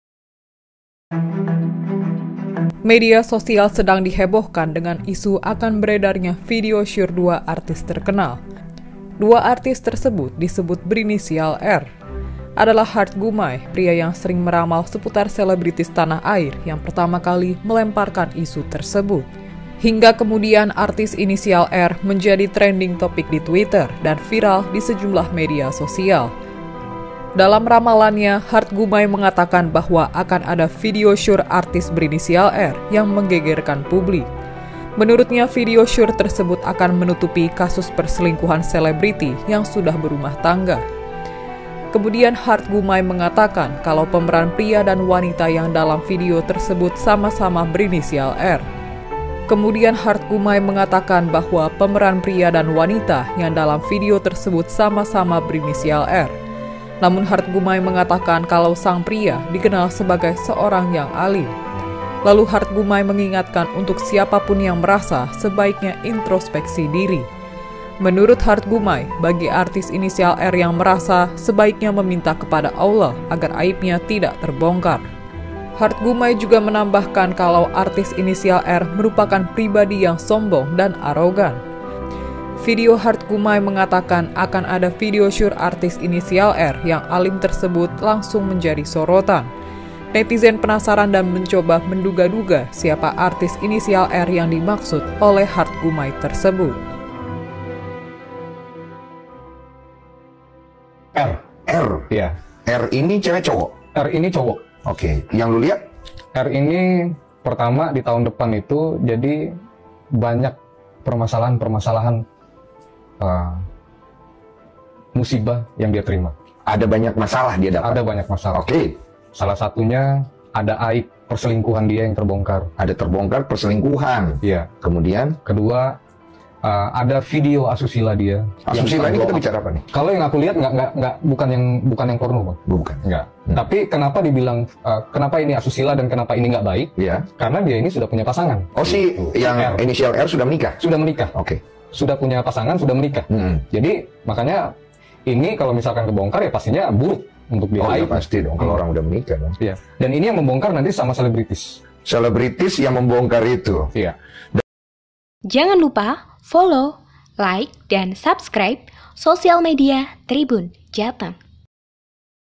Viral Prediksi Hard Gumay Video Syur Artis Inisial R Selingkuh
Adalah Hard Gumay, pria yang sering meramal seputar selebritis tanah air yang pertama kali melemparkan isu tersebut.